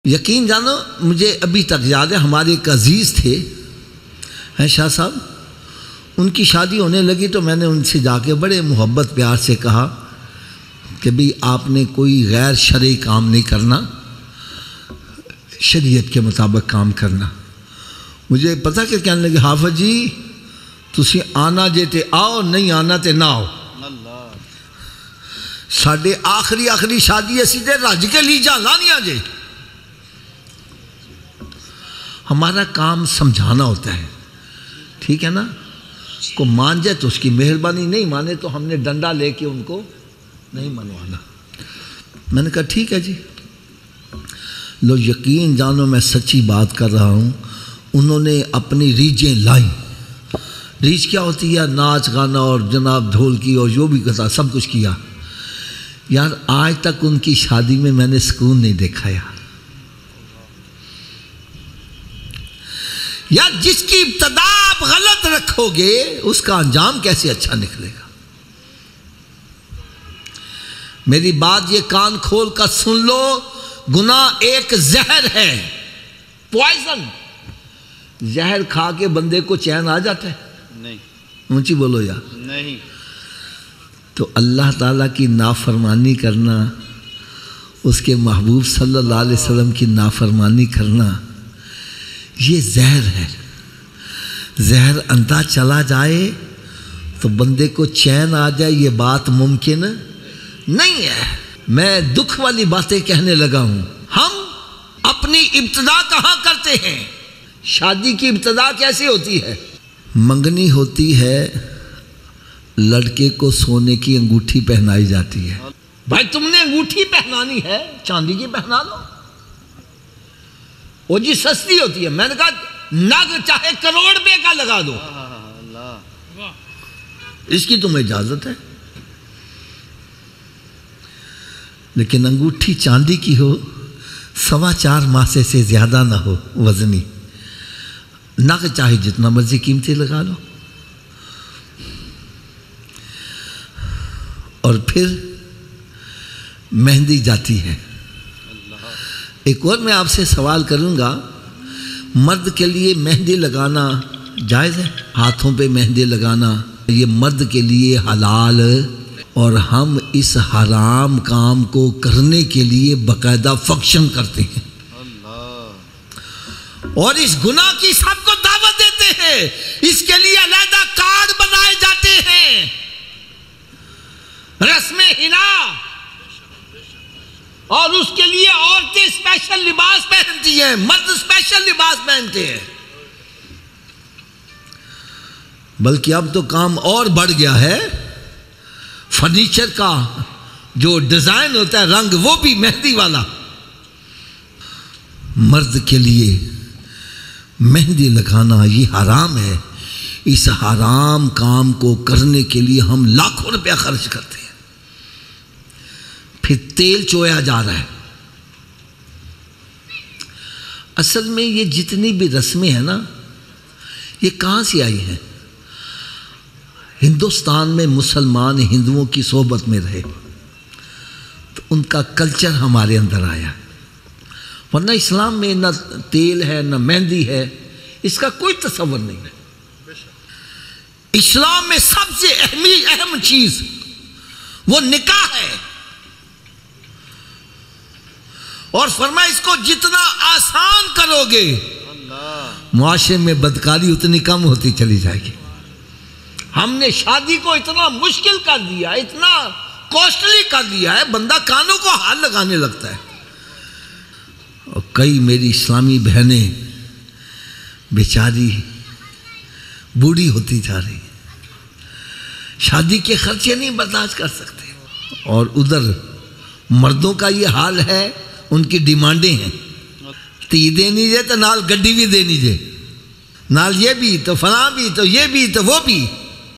Sone Ki Anguthi | Shadi Bayah ki Ghalat Rasoomat By Hafiz Hafeez Ur Rehman Qadri 2022
Sone Ki Anguthi | Shadi Me Ghalat Rasoomat By Hafiz Hafeez Ur Rehman Qadri 2022

Live 4 clip sone ki angothi - ghalt rasomat bismillah
 ABOUT VIDEO: Dekhiye Islah e Muashra ki niyyat se khobsoorat aur maloomati short video

►Share This Beautiful Bayan , Hamd , & Naats On Ur Facebook,Twitter For Sawab e Jaria

●▬▬▬▬▬▬▬▬▬▬▬ஜ۩۞۩ஜ▬▬▬▬▬▬▬▬▬▬▬●